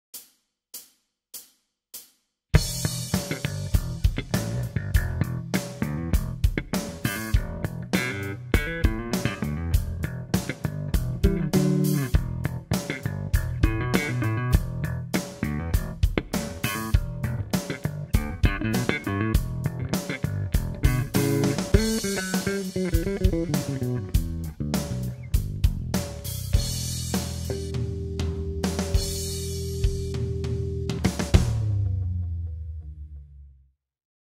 Slap rock bass
base de slap utilizando la escala pentatónica menor y el modo dórico de E